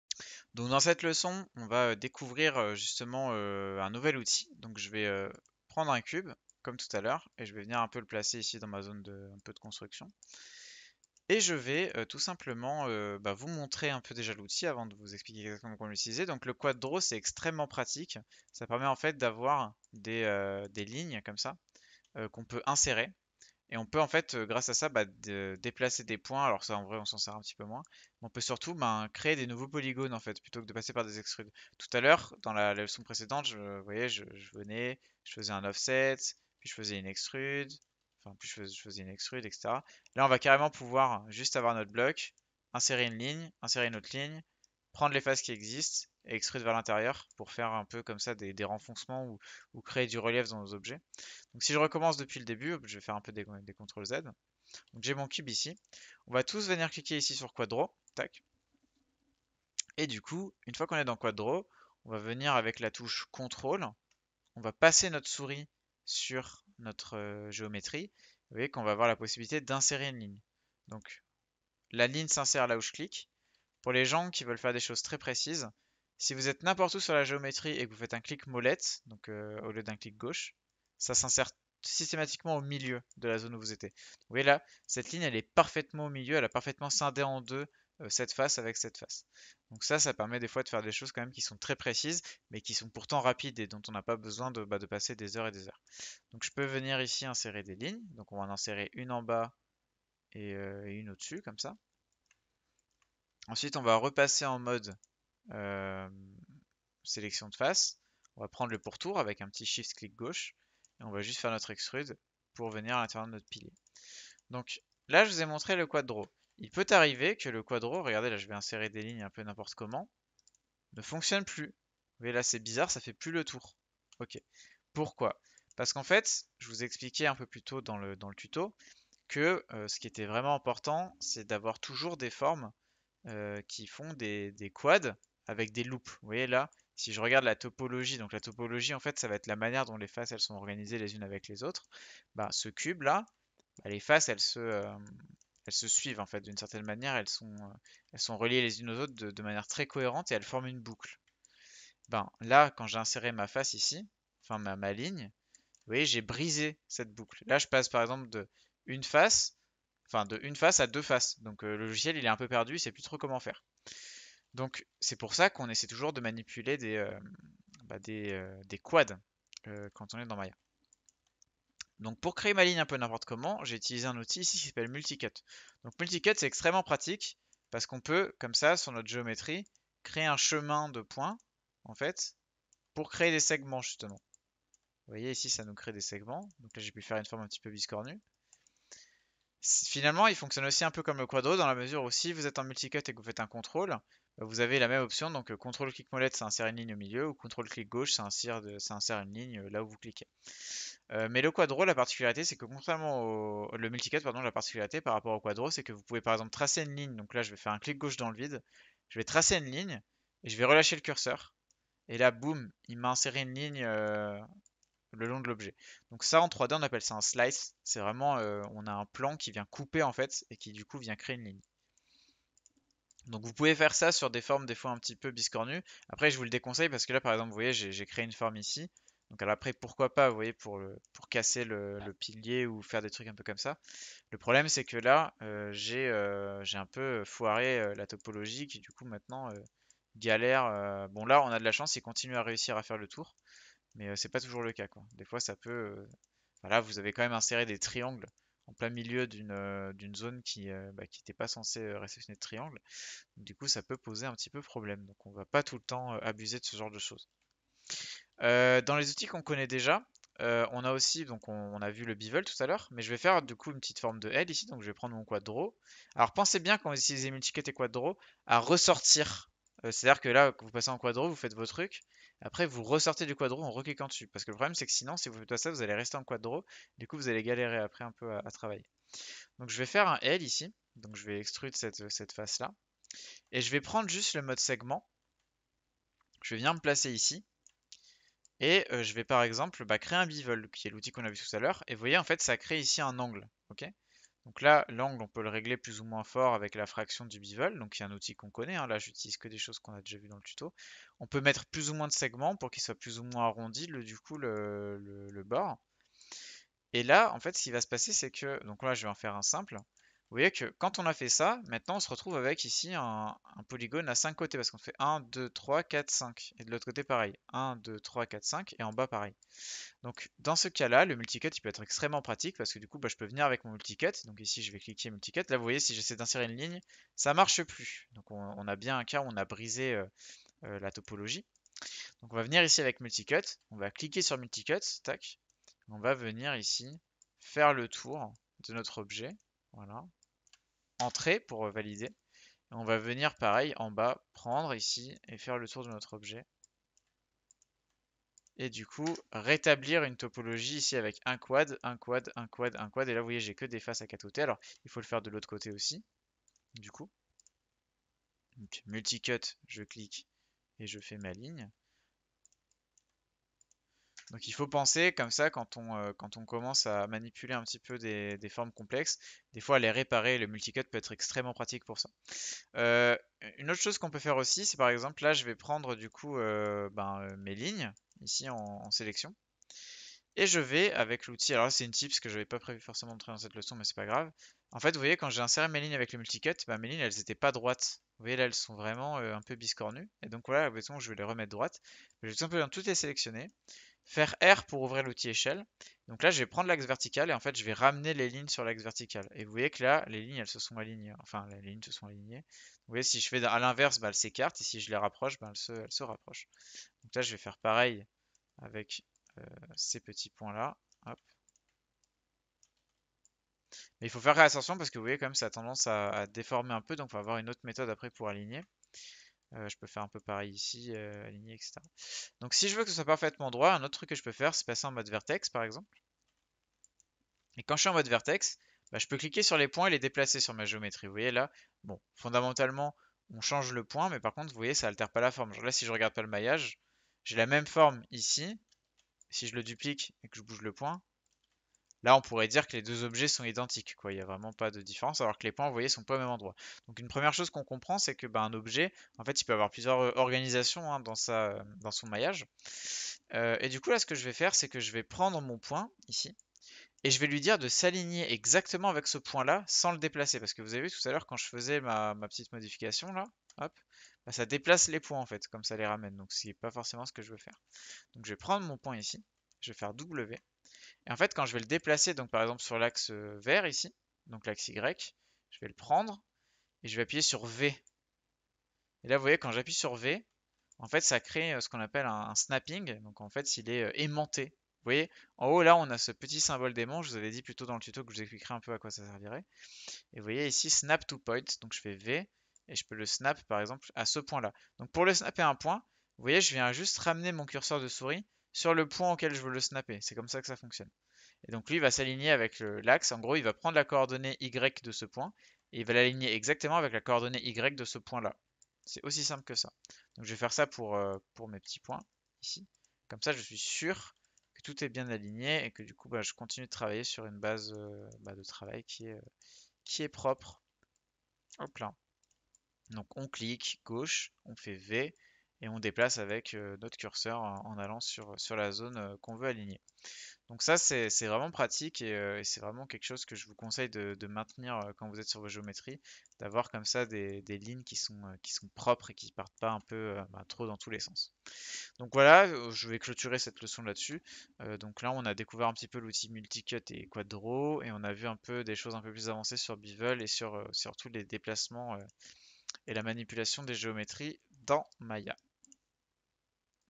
Maya : Quad Draw, Multi-Cut, Edge Loops, Snap de Vertex et Weld
Dans ce tutoriel, découvrez comment maîtriser des outils clés de Maya pour la modélisation 3D : Quad Draw, Multi-Cut, insertion d’edge loops, ainsi que les outils de snap de vertex et weld. Apprenez à créer des géométries précises, assembler vos modèles efficacement et perfectionner vos workflows pour des projets en 3D.

Ce que vous apprendrez :

    Création et modification avec Quad Draw et Multi-Cut
    Insertion d’edge loops pour affiner la géométrie
    Utilisation des outils de snap de vertex et weld pour assembler vos modèles
    Techniques pour un workflow optimisé en modélisation avancée